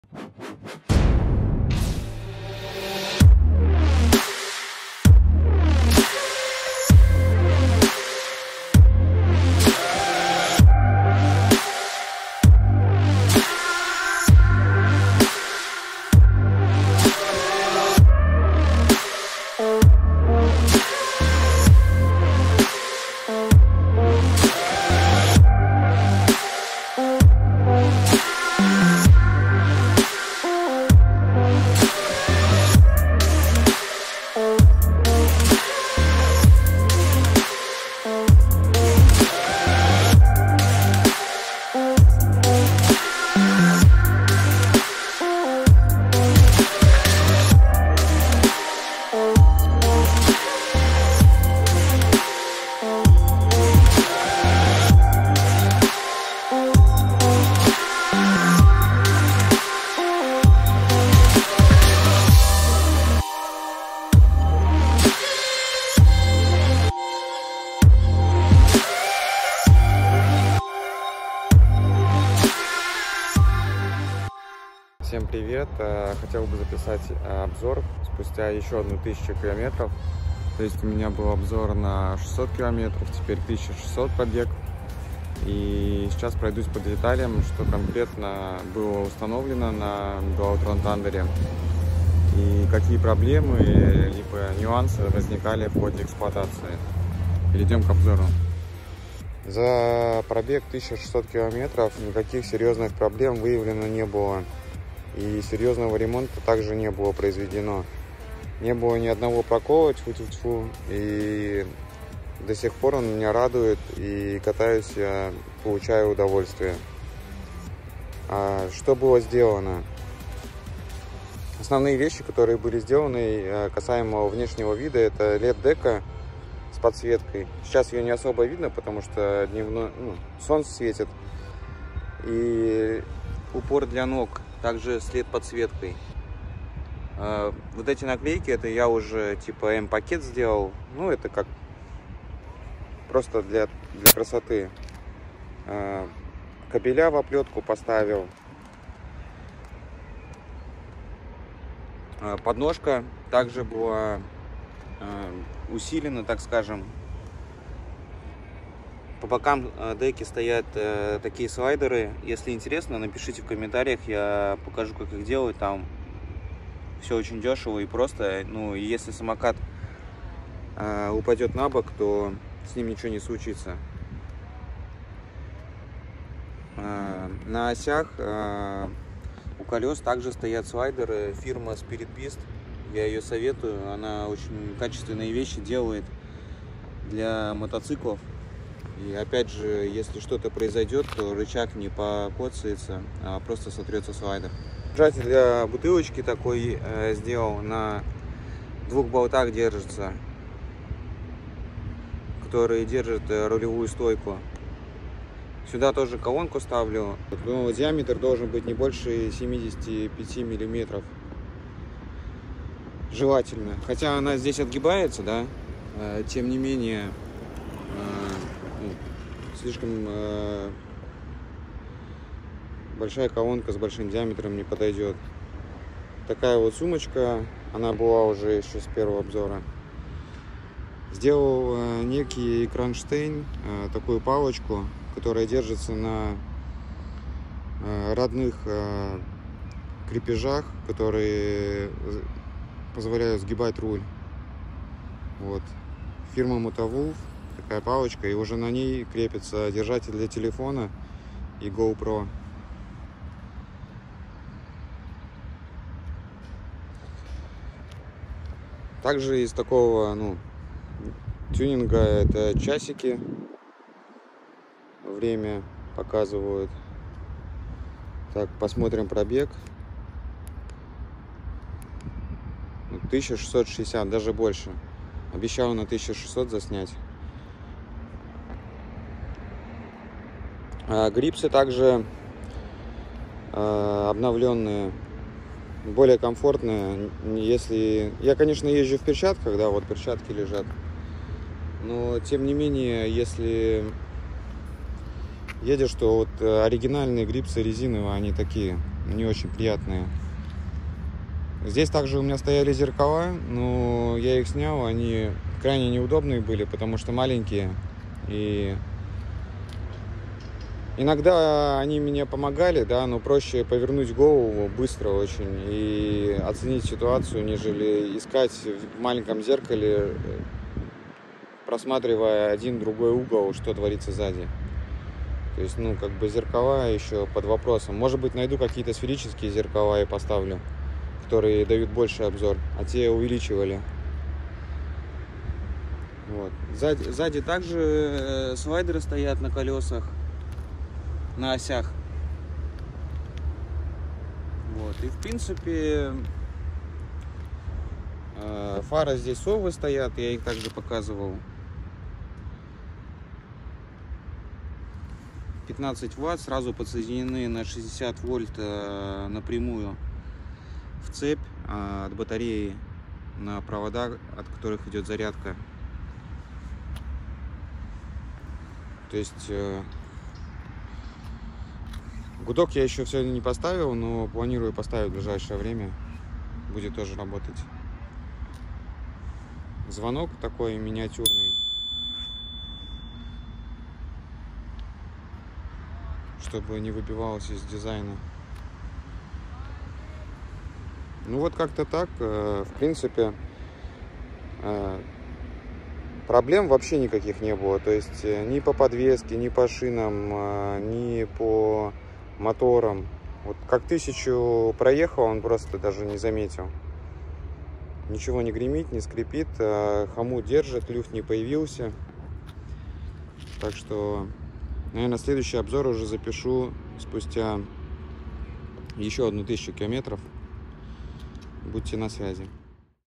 Dualtron Thunder спустя 1600км. Тюнинг и обзор.
Проехал еще 1000 км после последнего обзора и в общей сумме 1600 км, поломок и проколов не было.